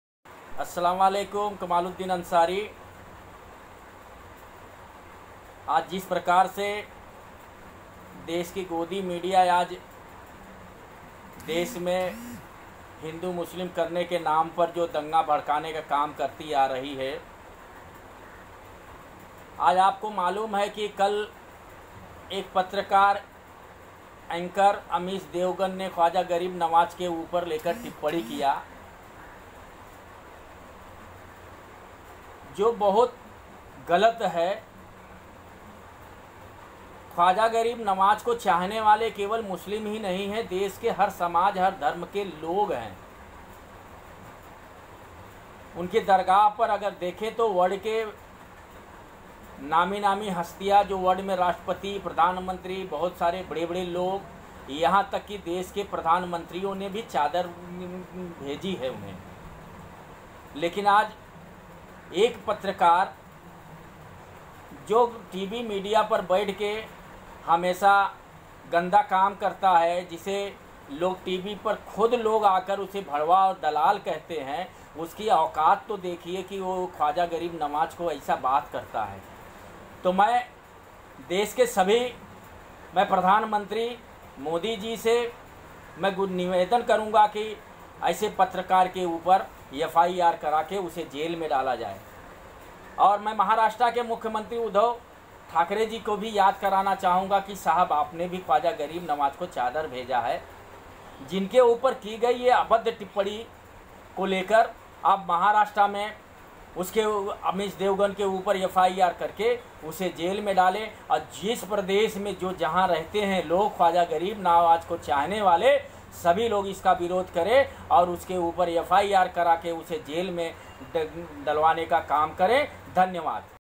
ख्वाजा गरीब नवाज पर मीडिया एंकर अमिष देवगन द्वारा अभद्र टिप्पड़ी को लेकर हमारी माँग
ख्वाजा गरीब नवाज पर की गई अभद्र टिप्पड़ी पर देश के प्रधानमंत्री नरेंद्र मोदी जी व महाराष्ट्र के मुख्यमंत्री उद्धव साहेब ठाकरे जी से माँग करता हूँ अमिष देवगन पर तुरन्त कार्रवाई करके जेल में डाला जाए।